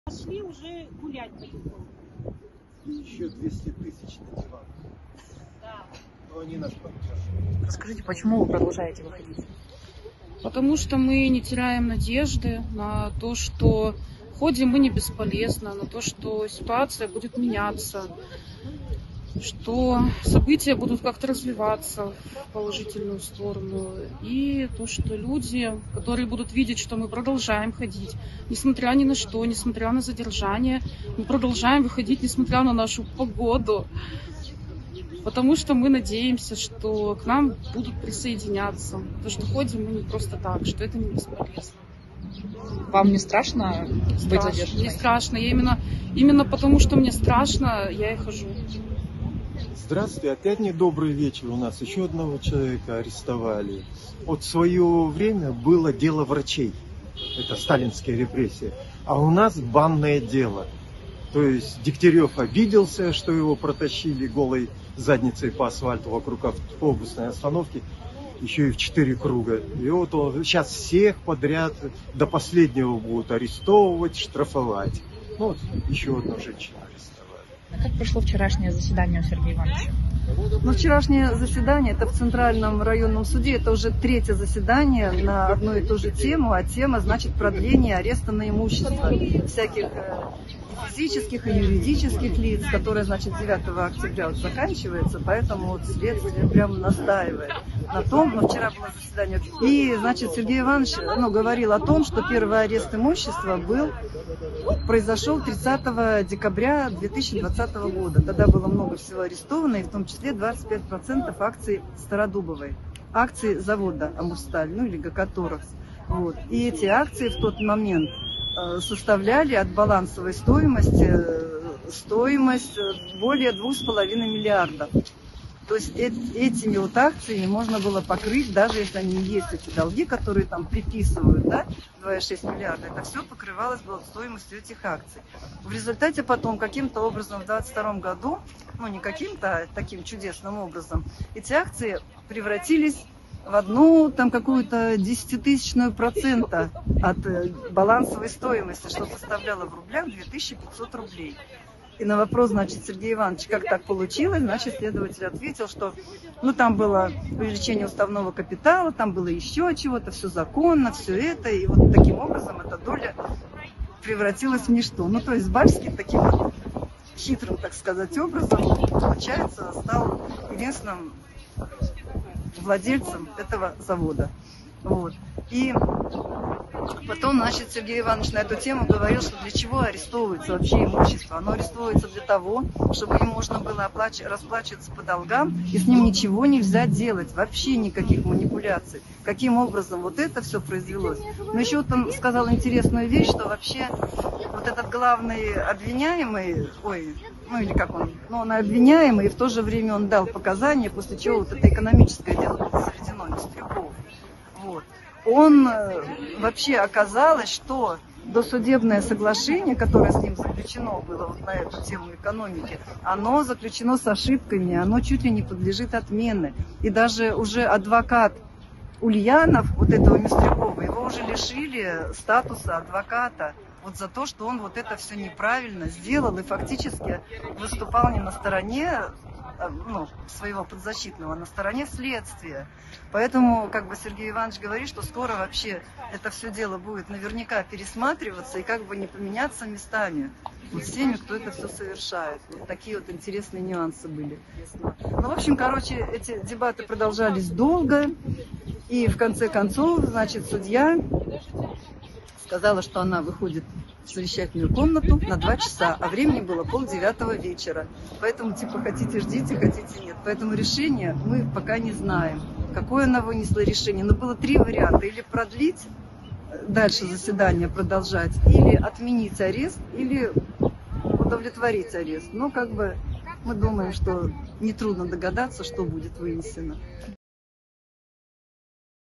🟠486-й день Бессрочного пикета в Хабаровске
"Именно потому что мне страшно, я и выхожу на пикет"

486-й день Бессрочного пикета в Хабаровске в защиту Сергея Фургала и всех политзаключённых.

Задержана Тамара, по ч. 5 статьи 20.2 за акцию 18 сентября.